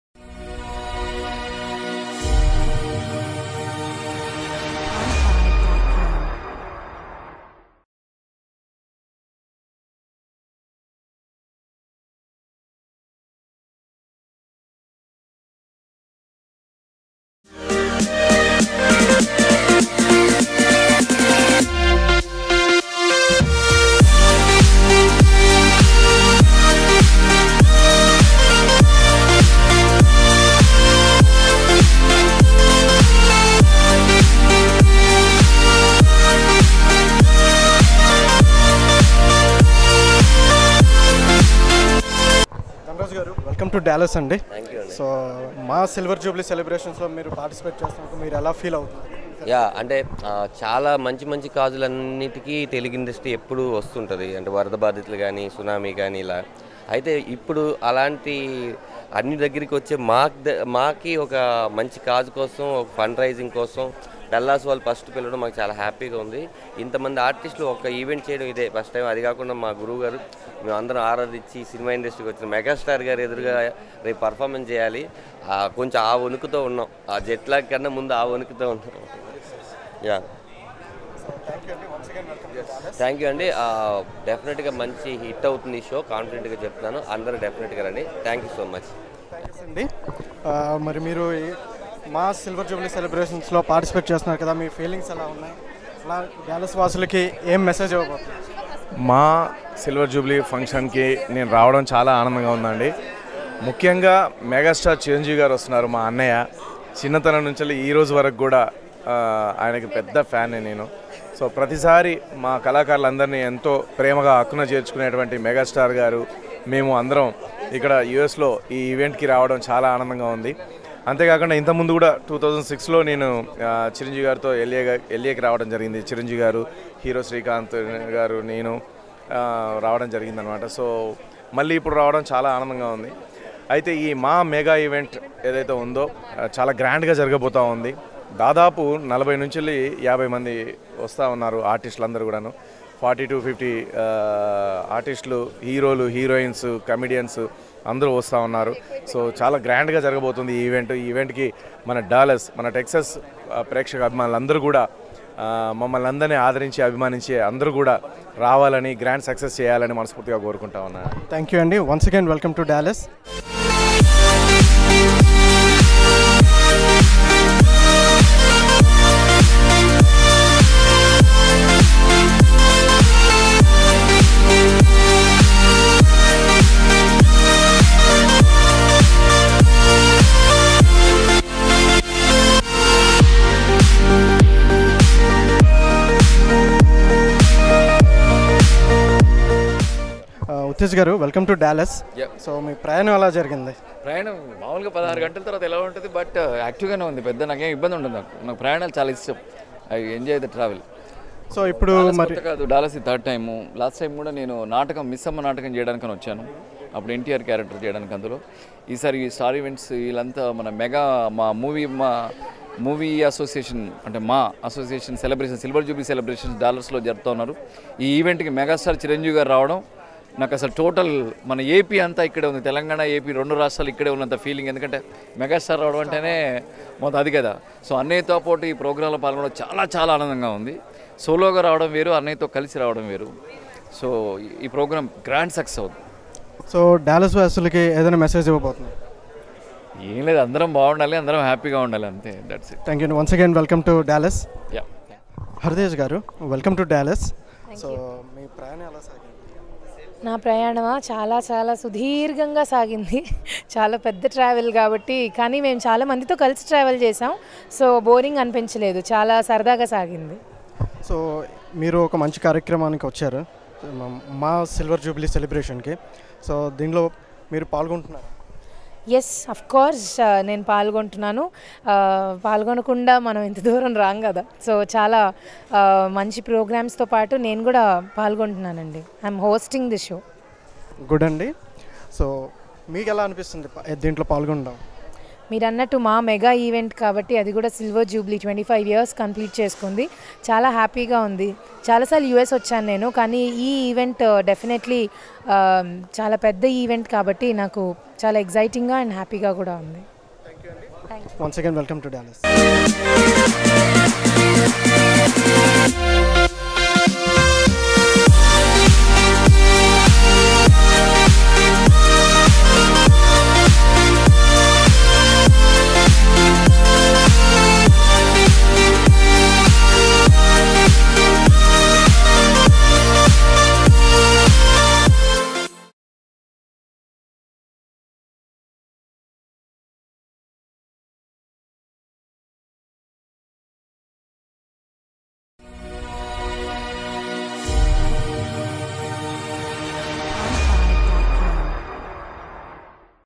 Chiranjeevi Mega Event - MAA STARS arrive at Airport in Dallas
Chiranjeevi Mega  Event - MAA STARS arrive at Airport in Dallas| DesiplazaTV.

BANQUET NIGHT - MAA STAR EVENTS.